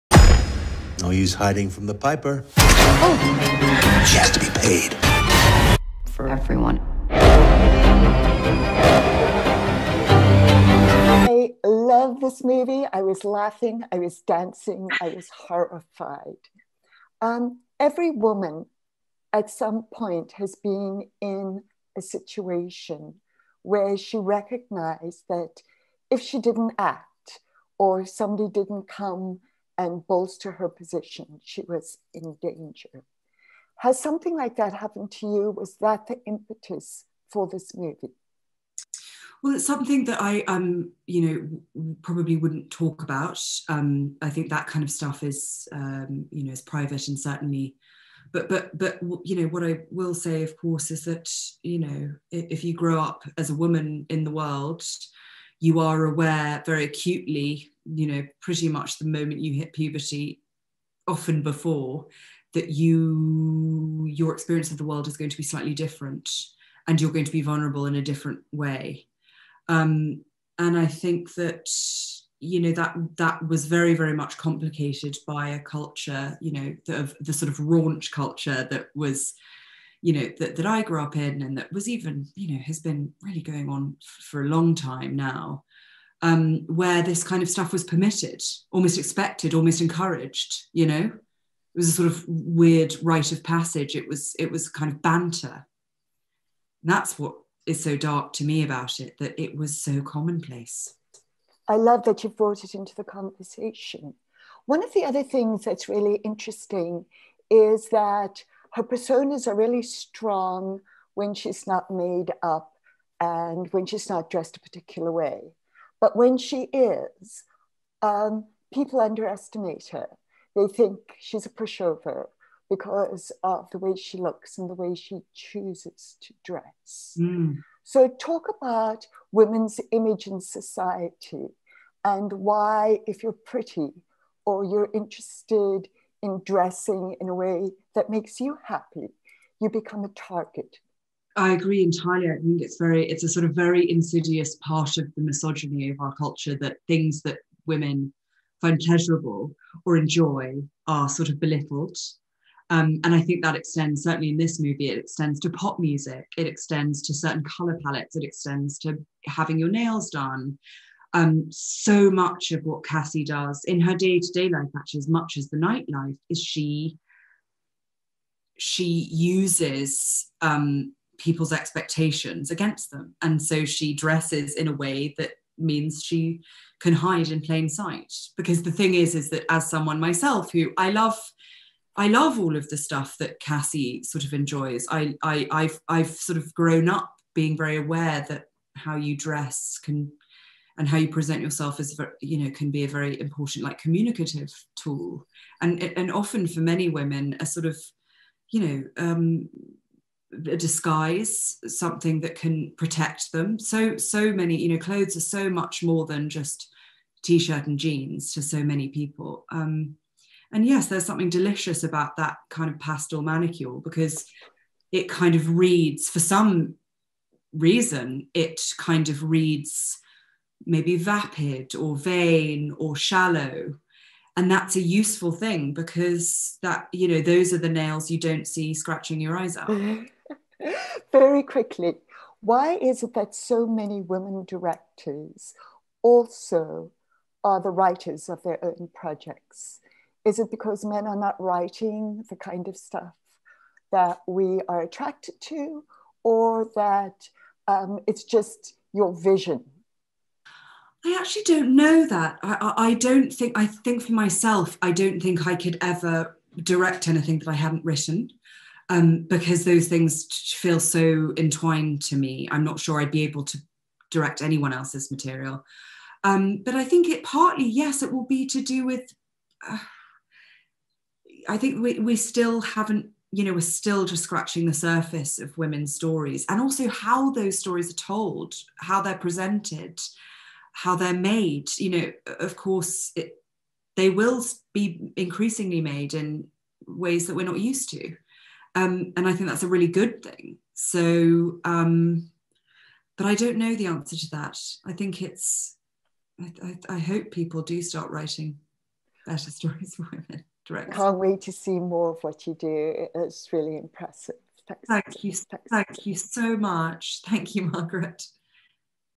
Director, Emerald Fennell, chats to Margaret Gardiner about her new film
Go behind-the-scenes and in-depth with the director of the deliciously twisted new revenge thriller, PromisingYoungWoman, starring Carey Mulligan.

And don't forget to catch it in cinemas NOW!